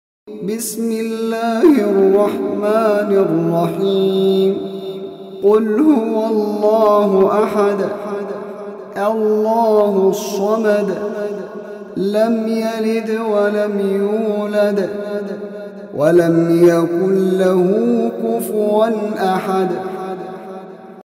صبح شام تین تین بار سورۃ اخلاص سورۃ الناس سورۃ الفلق کی تلاوت کرنےوالا ہر آفت سے محفوظ رھے گا
حضور صل اللہ علیہ وآلہ وسلم نے فرمایا سورۃ الناس سورۃ اخلاص سورۃ الفلق کی تلاوت  صبح شام تین تین بار کرنے والا ہر آفت سے محفوظ  رھے گا